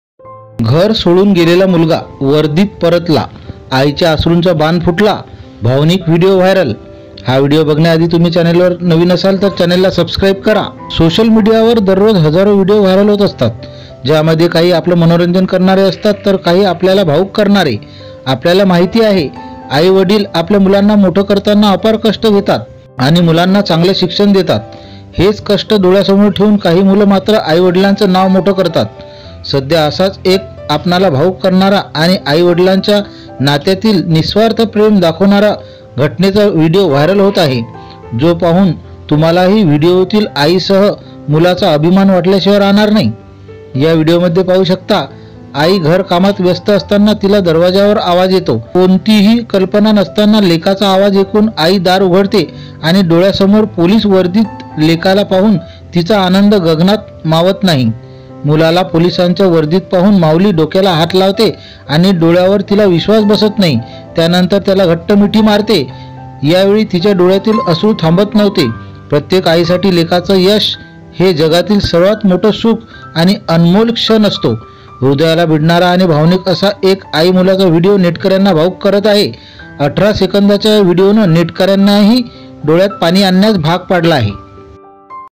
घर सोडून गेलेला मुलगा वर्दीत परतला आईच्या अश्रूंचा बांध फुटला पाहा हा भावनिक व्हिडीओ
News Related topic 👇

VM Marathi news
maharashtra
Maharashtra news
Zee 24 tass